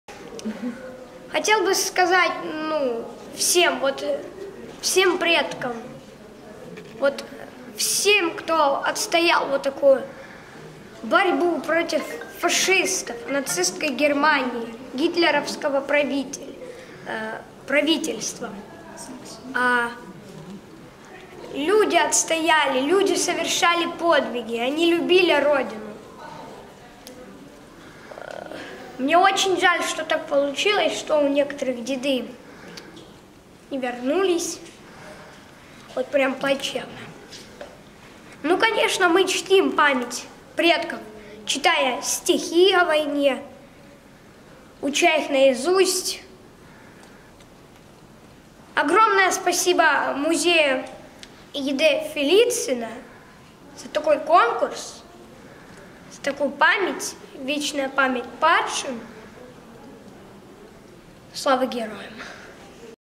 Эльдар Алиев - Конкурс чтецов 2019 в Музее Фелицына
20 февраля в Краснодарском государственном историко-археологическом музее-заповеднике им. Е. Д. Фелицына состоялся конкурс чтецов стихов о войне «Победители».